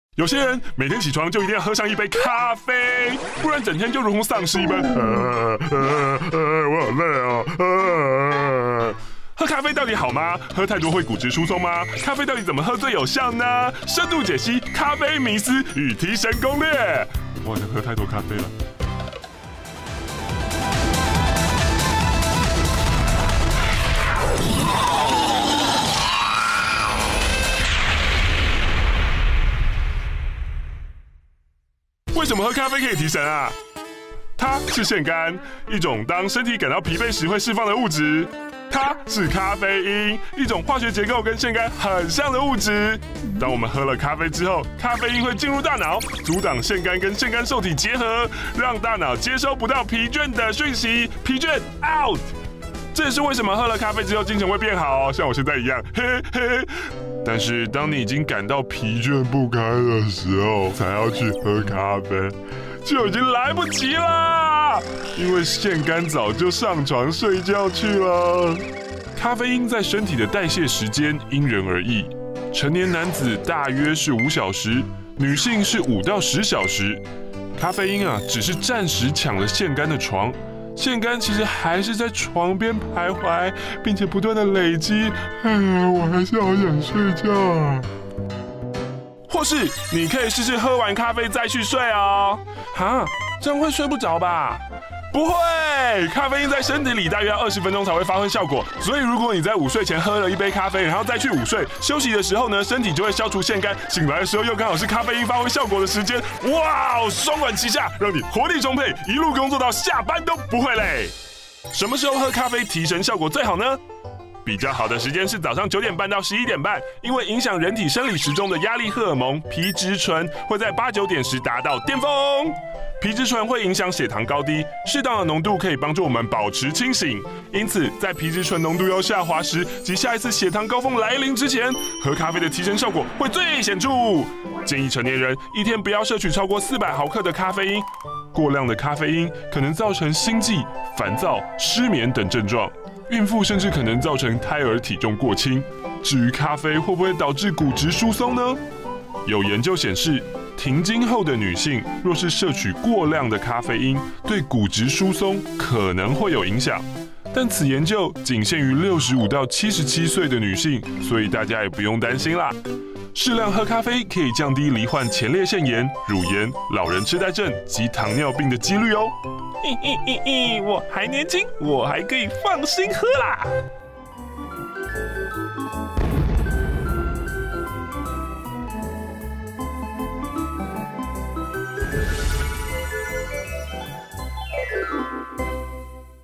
咖啡迷思與提神攻略！｜科學大爆炸EP.45
早上起床一杯咖啡……吃完午餐一杯咖啡……精神不好一杯咖啡……每天都要喝好多好多咖啡的你，知道為什麼喝咖啡可以提神嗎？挑對時間還能讓你更有精神喔！

延伸閱讀：
咖啡因究竟對身體有何影響？一天喝三杯會不會太多？
牛頓冷卻定律：想來一杯溫度完美的咖啡？先學好微分方程吧！──《超展開數學約會》